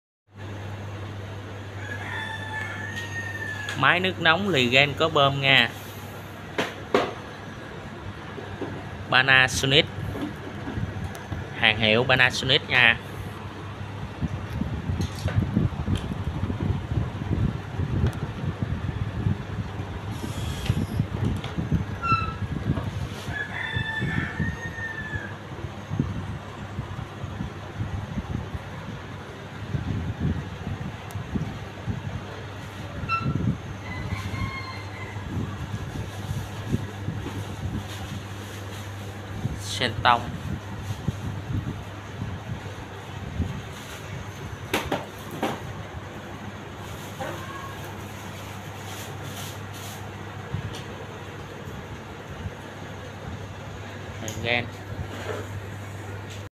lô máy nước nóng siêu an toàn Malaysia
lô máy thanh lý chất lượng cao hàng Malaysia centon.. Panasonic Legend
không bơm và có bơm trợ lực khi nước yếu công nghệ chống giật siêu an toàn quốc công ty bảo hành uy tín
Zalo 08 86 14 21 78 OBC